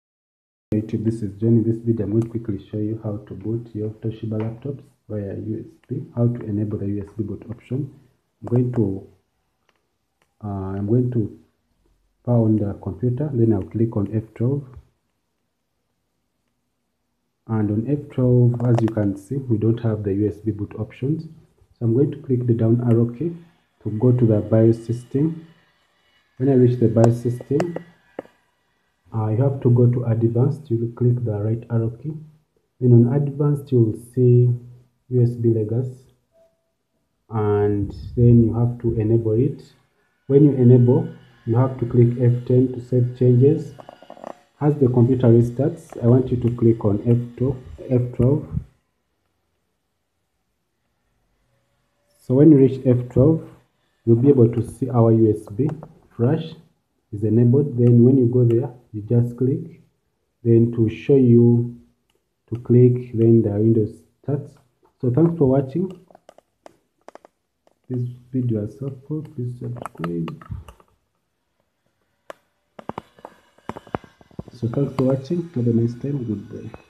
How to Enable Usb Boot Options On Toshiba
How do I get my Toshiba laptop to boot from USB?
To boot this drive after many hours of trying, sadly only because of a bug:

    1.Reset your bios to default settings!
    2. Leave Secureboot ON.
    3. Turn Fastboot OFF.
    4. Save settings with F10.
    5. Hold F12 while booting to get to the boot menu, and select your USB.
    6. It should boot!

Toshiba How-To: Setting The System Boot Priority in Bios